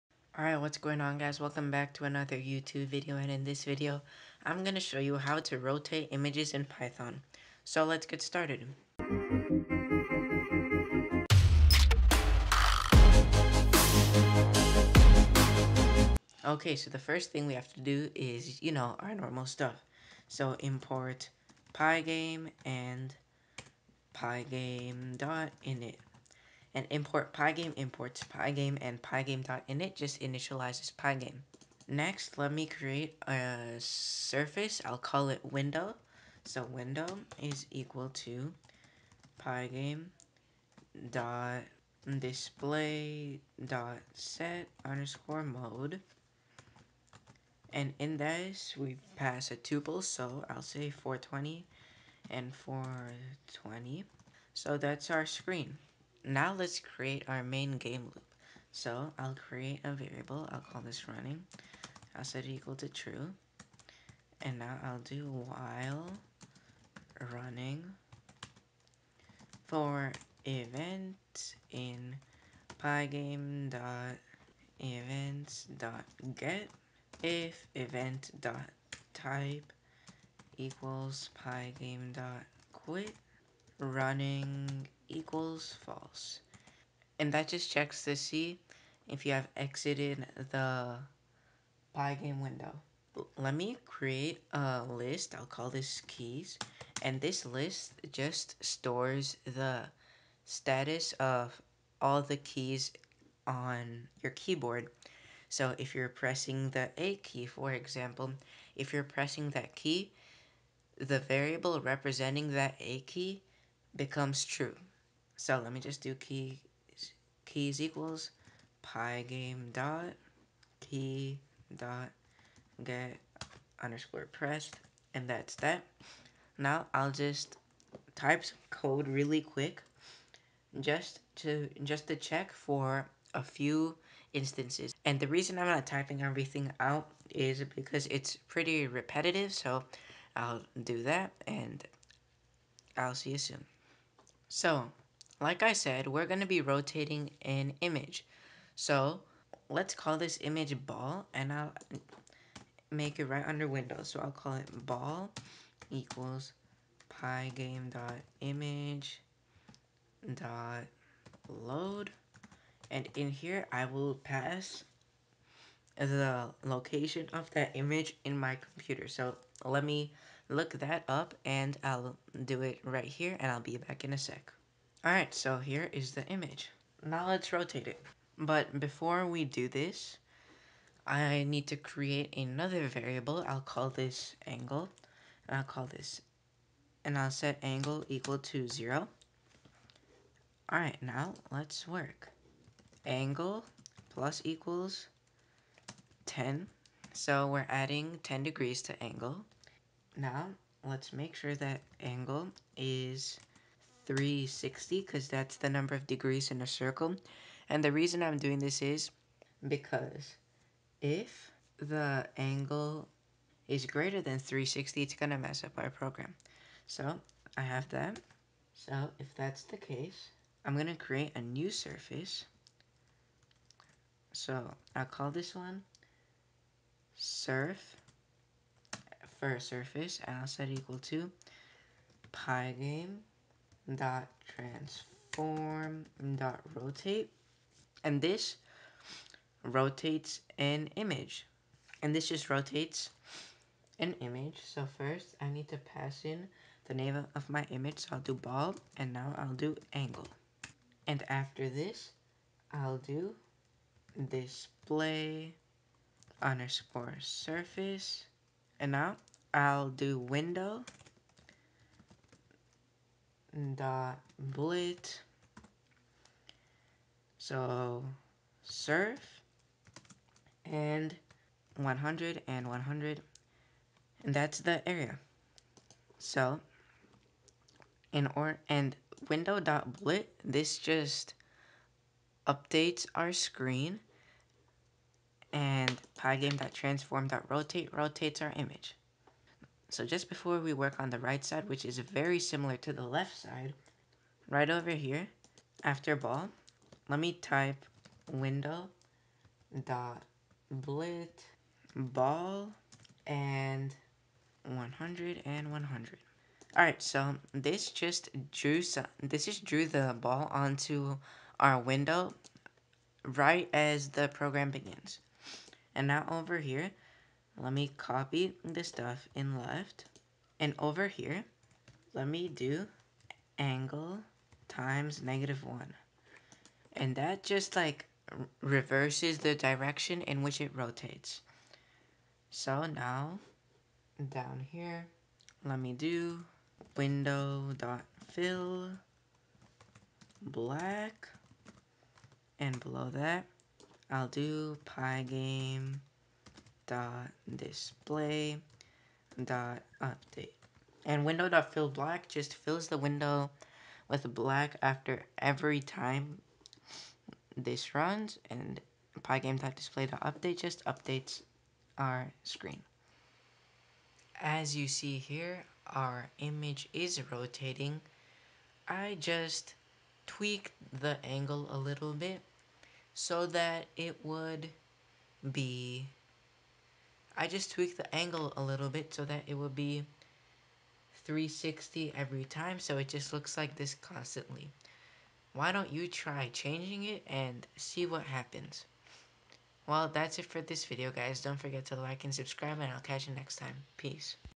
How To Rotate Images In Pygame
In this video I show you how to rotate images in Pygame!